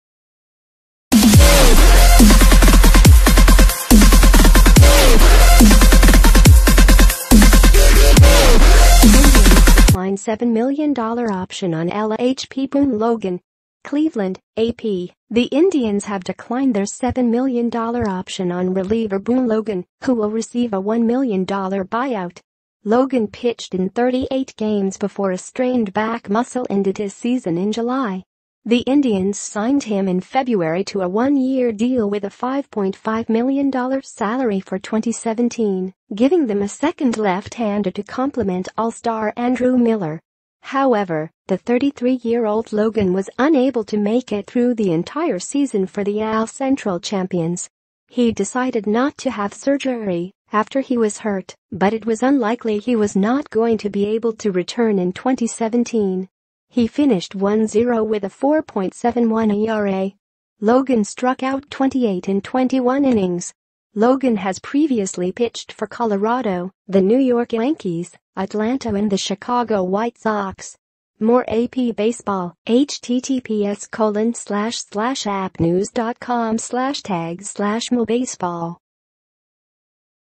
Indians decline $7 million option on LHP Boone Logan
CLEVELAND (AP) — The Indians have declined their $7 million option on reliever Boone Logan, who will receive a $1 million buyout.

Logan pitched in 38 games before a strained back muscle ended his season in July. The Indians signed him in February to a one-year deal with a $5.5 million salary for 2017, giving them a second left-hander to complement All-Star Andrew Miller.

However, the 33-year-old Logan was unable to make it through the entire season for the AL Central champions. He decided not to have surgery after he was hurt, but it was unlikely he was not going to be able to return in 2017.